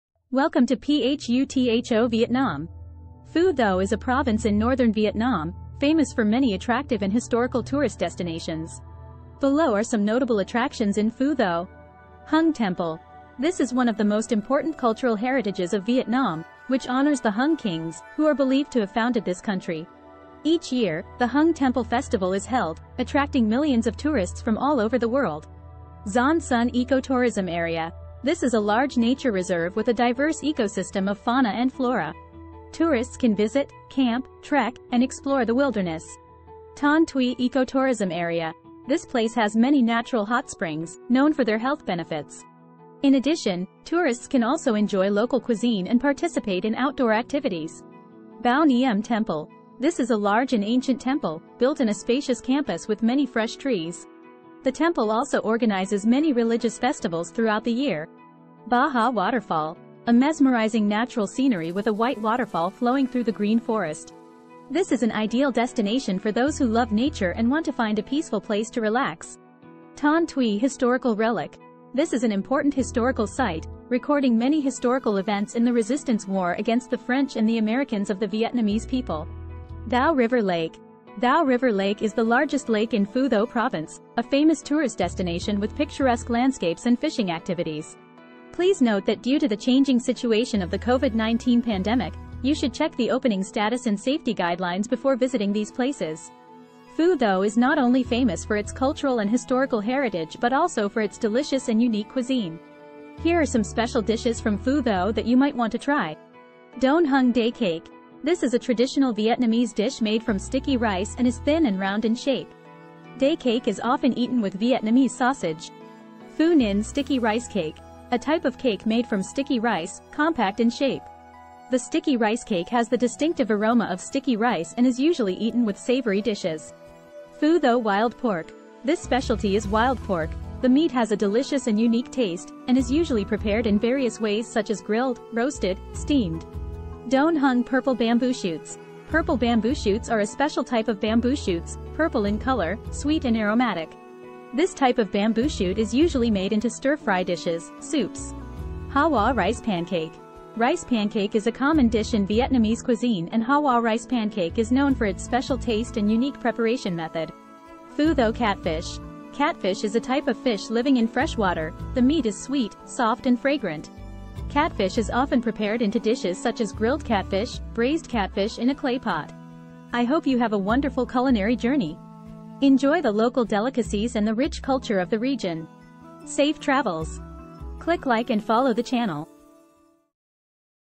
TOURISM ANDFOODIN-PHUTHO-VIETNAM
WELCOME TO PHUTHO-VIETNAM

Phu Tho is a province in Northern Vietnam, famous for many attractive and historical tourist destinations.

Below are some notable attractions in Phu Tho:

Hung Temple: This is one of the most important cultural heritages of Vietnam, which honors the Hung Kings, who are believed to have founded this country.

Each year, the Hung Temple Festival is held, attracting millions of tourists from all over the world.

Xuan Son Ecotourism Area: This is a large nature reserve with a diverse ecosystem of fauna and flora.

Tourists can visit, camp, trek, and explore the wilderness.

Thanh Thuy Ecotourism Area: This place has many natural hot springs, known for their health benefits.

In addition, tourists can also enjoy local cuisine and participate in outdoor activities.

Bao Nghiem Temple: This is a large and ancient temple, built in a spacious campus with many fresh trees.

 The temple also organizes many religious festivals throughout the year.

Ba Ha Waterfall: A mesmerizing natural scenery with a white waterfall flowing through the green forest.

This is an ideal destination for those who love nature and want to find a peaceful place to relax.

Thanh Thuy Historical Relic: This is an important historical site, recording many historical events in the resistance war against the French and the Americans of the Vietnamese people.

Thao River Lake: Thao River Lake is the largest lake in Phu Tho province, a famous tourist destination with picturesque landscapes and fishing activities.

Please note that due to the changing situation of the Covid-19 pandemic, you should check the opening status and safety guidelines before visiting these places.

Phu Tho is not only famous for its cultural and historical heritage but also for its delicious and unique cuisine.

Here are some special dishes from Phu Tho that you might want to try:

Doan Hung Day Cake: This is a traditional Vietnamese dish made from sticky rice and is thin and round in shape.

Day Cake is often eaten with Vietnamese sausage.

Phu Ninh Sticky Rice Cake: A type of cake made from sticky rice, compact in shape.

The Sticky Rice Cake has the distinctive aroma of sticky rice and is usually eaten with savory dishes.

Phu Tho Wild Pork: This specialty is wild pork, the meat has a delicious and unique taste, and is usually prepared in various ways such as grilled, roasted, steamed...

Doan Hung Purple Bamboo Shoots: Purple bamboo shoots are a special type of bamboo shoots, purple in color, sweet and aromatic.

This type of bamboo shoot is usually made into stir-fry dishes, soups...

Ha Hoa Rice Pancake: Rice pancake is a common dish in Vietnamese cuisine and Ha Hoa rice pancake is known for its special taste and unique preparation method.

Phu Tho Catfish: Catfish is a type of fish living in freshwater, the meat is sweet, soft and fragrant.

 Catfish is often prepared into dishes such as grilled catfish, braised catfish in a clay pot...

Remember to check the pandemic situation before you decide to visit and enjoy the cuisine at restaurants or eateries.

Click like and follow the channel